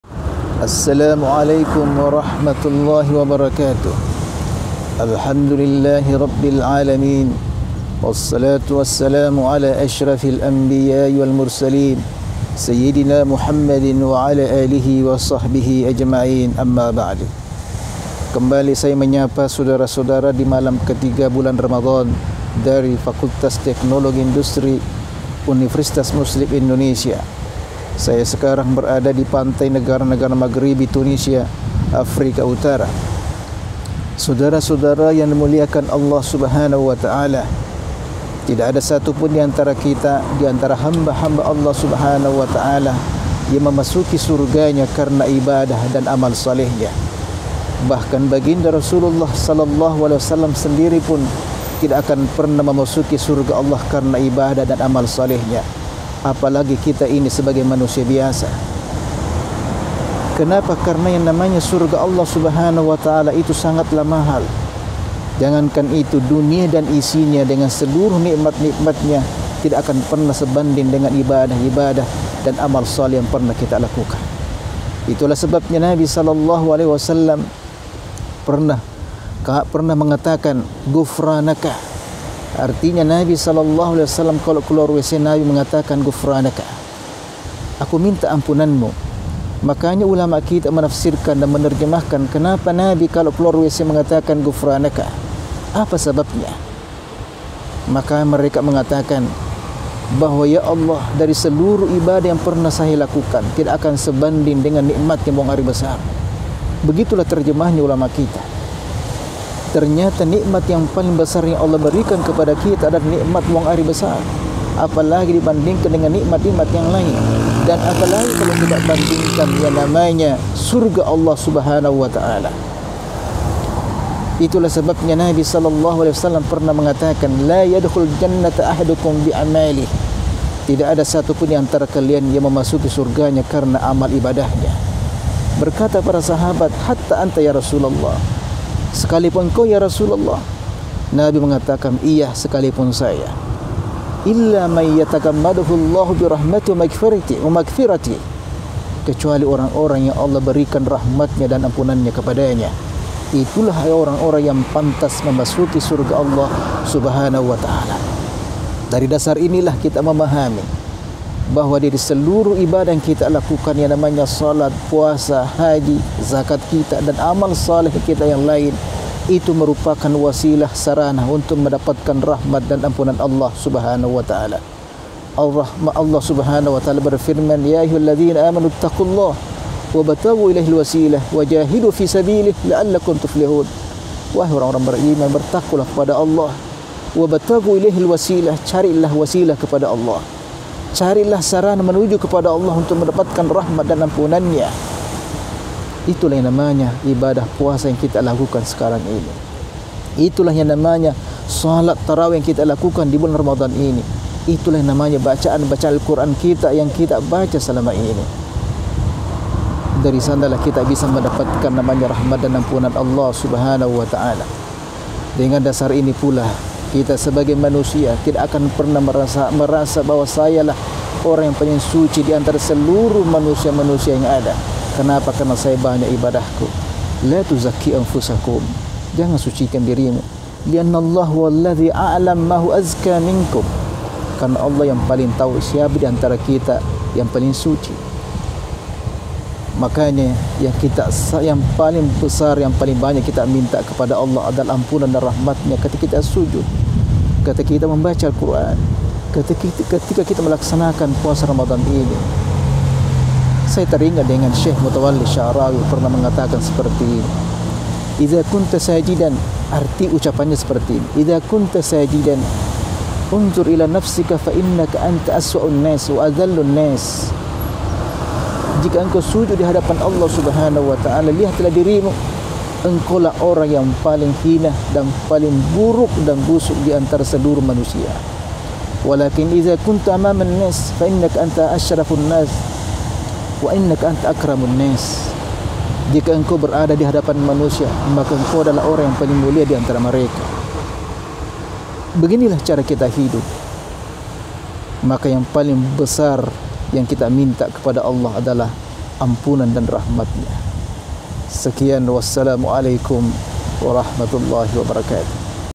#3 Ramadan “Tidak Masuk Surga Seseorang Hanya Karena Amal Ibadahnya”oleh: Dr H Yusri Muhammad Arsyad
Oleh Dr. H. Yusri Muhammad Arsyad, Lc., MA
(Wakil Dekan 4 Fakultas Teknologi Industri UMI Makassar)